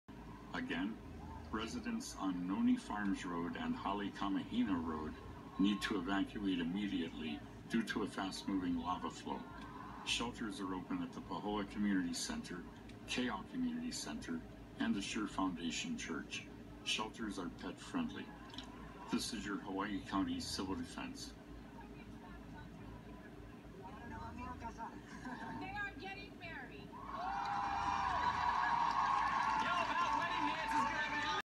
Lava Evacuation in Hawaii on TV
Mandatory evacuation for Leilani Estates Residents in Hawaii. Lava Fissures upward of 200ft. Fissure #8 re- opened.